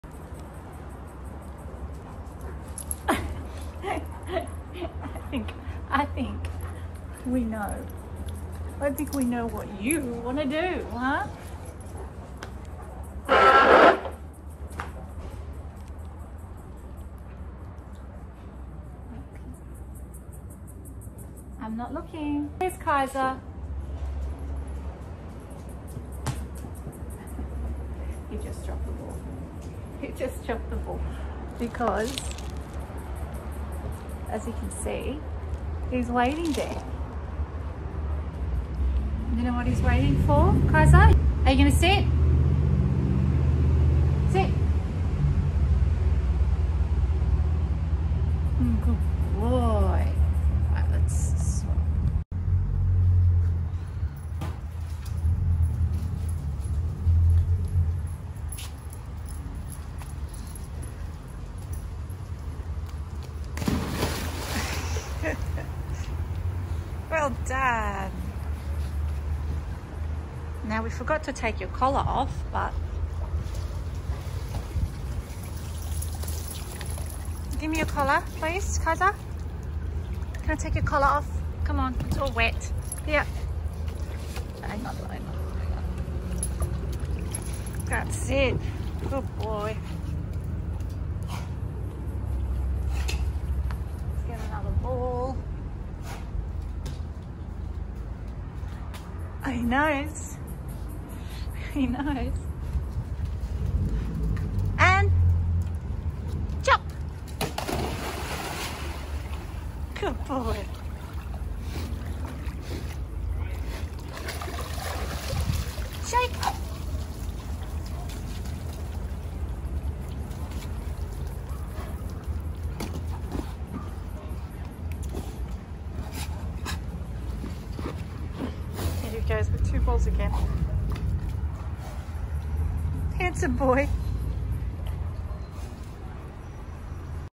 Kaiser the German shepherd gets owners attention to swim!
You can’t stop this guy from swimming! 💙